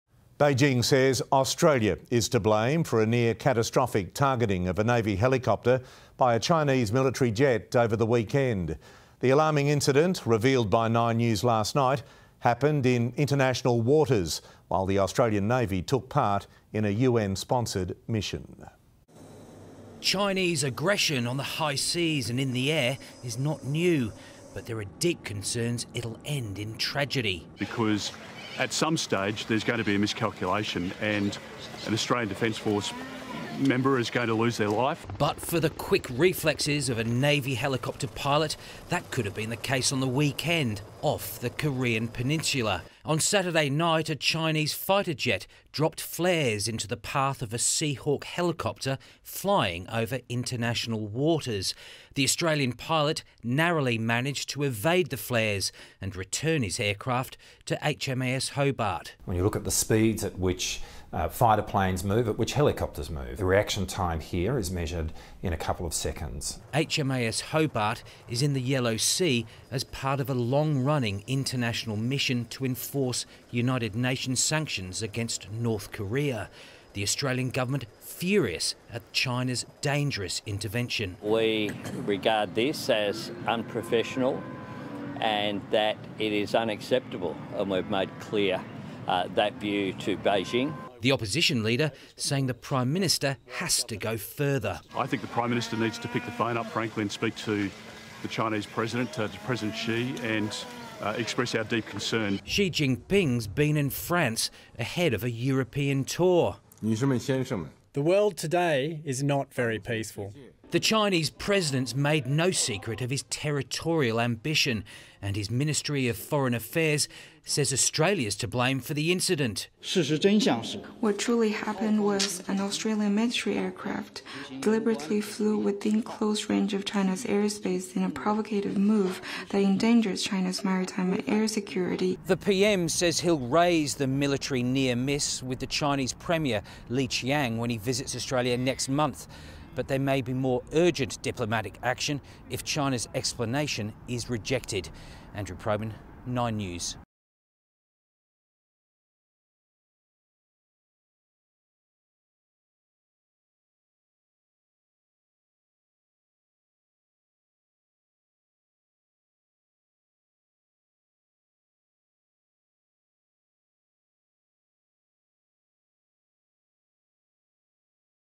PM Albanese urged to protest China’s President over targeting of navy helicopter | 9 News Australia
Prime Minister Anthony Albanese is being urged to communicate with China’s President to protest the targeting of navy helicopters by a Chinese Military jet.